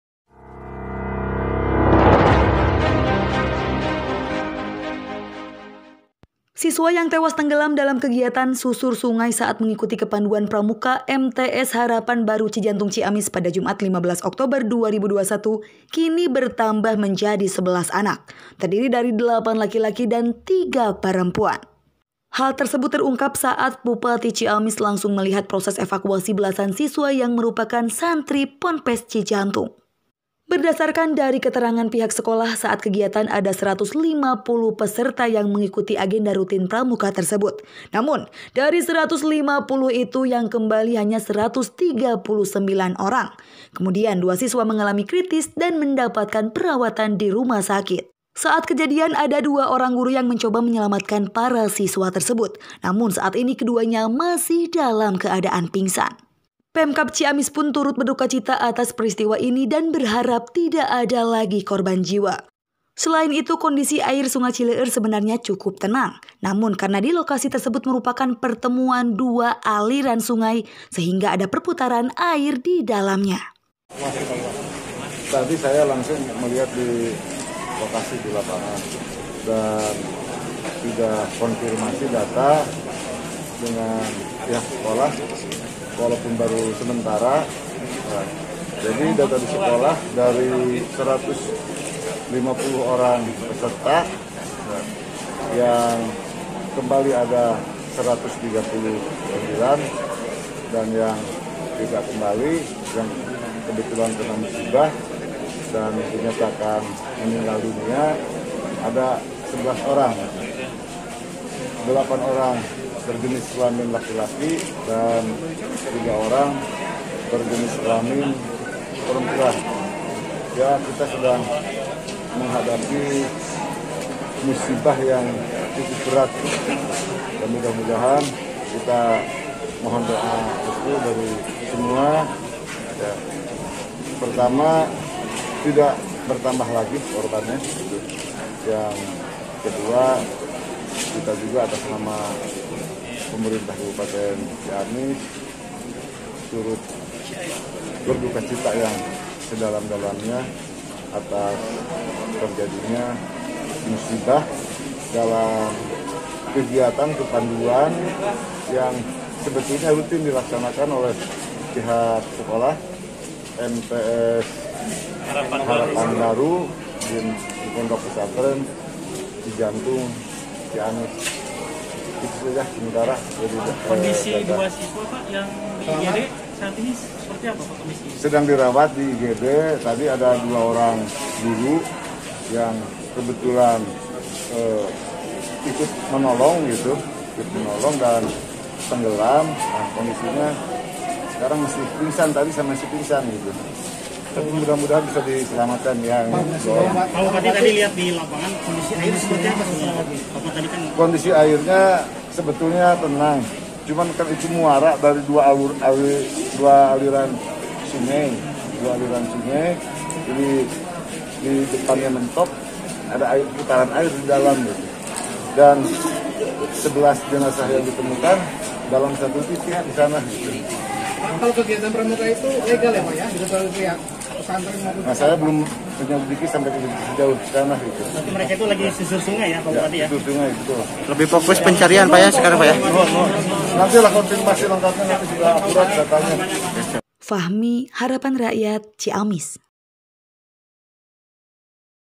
Siswa Tewas Tenggelam Jadi 11 Orang, Bupati Ciamis Berduka
Reporter: Fahmi
Editor: Muhafid
VO: Faudina